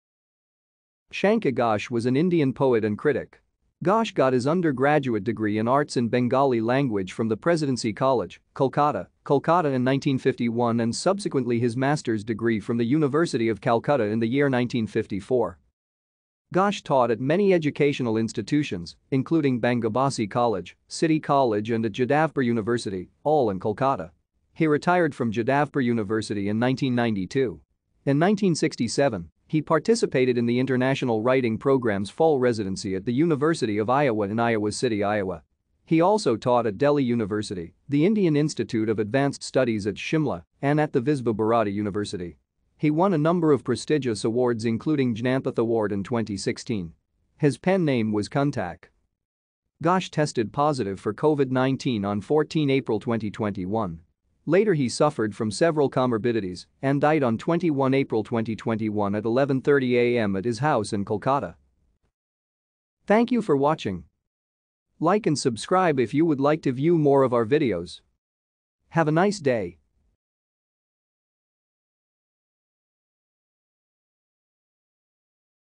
Shankha Ghosh
Shankha Ghosh  was an Indian poet and critic. Ghosh got his undergraduate degree in Arts in Bengali language from the Presidency College, Kolkata, Kolkata in 1951 and subsequently his master's degree from the University of Calcutta in the year 1954.